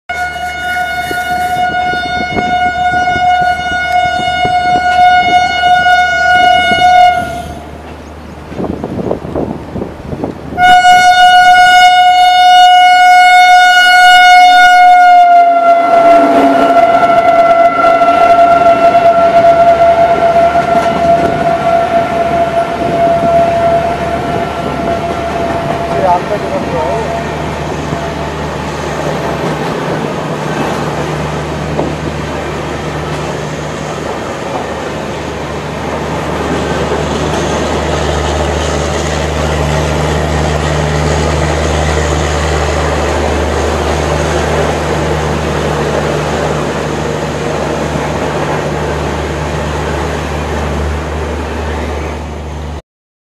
दूरोंतों एक्सप्रेस में लगे राजधानी एक्सप्रेस के कोच के साथ दूरोंतों एक्सप्रेस साहिबाबाद पार करते हुए
Rail Fan Me & Soni ji